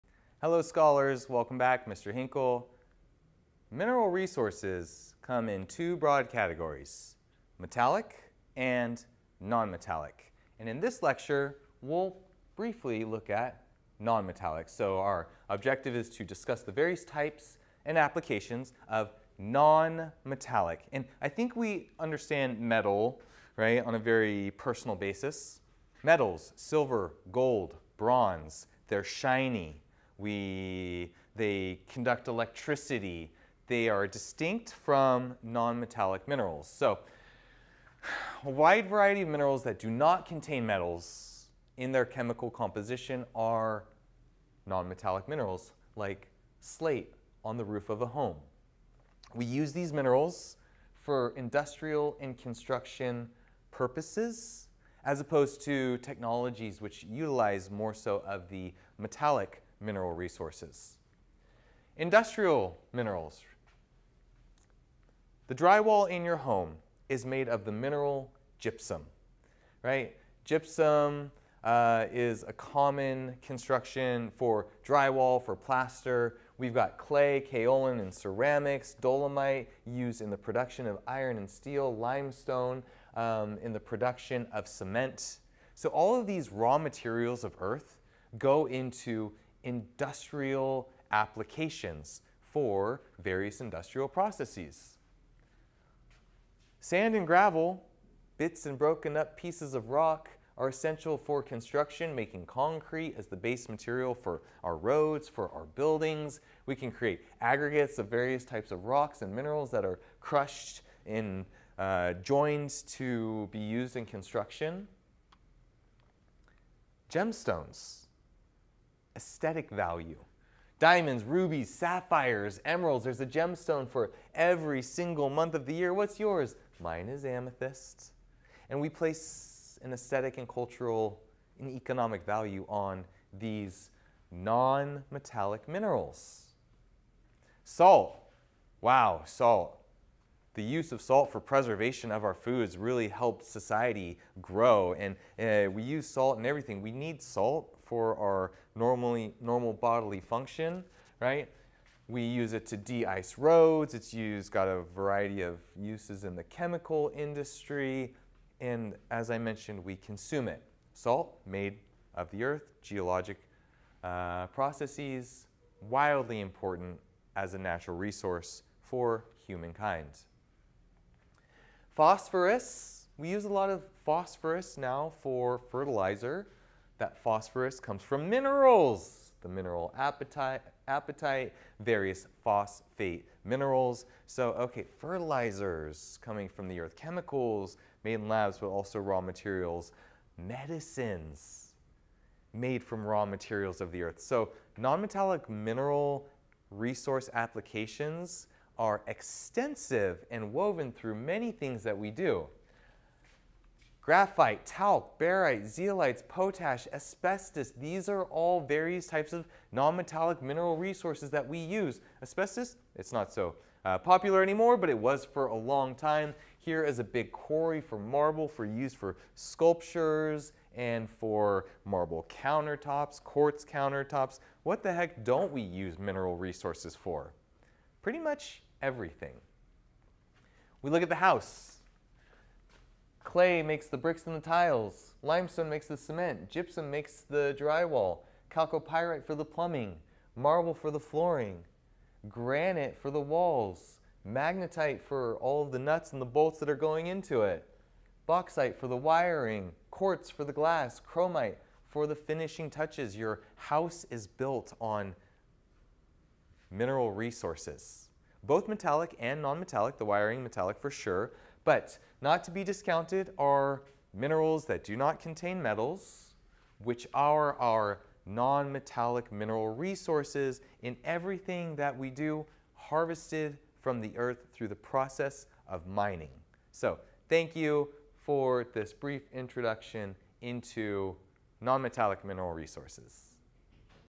Non-Metallic Mineral Deposits
Minerals are Earth materials and there are a wide variety of minerals that do not contain metals in their chemical composition. These minerals are often used for industrial and construction purposes due to their physical properties.

Objective: Discuss the various type and applications of non-metallic mineral deposits

Produced for Fresno City College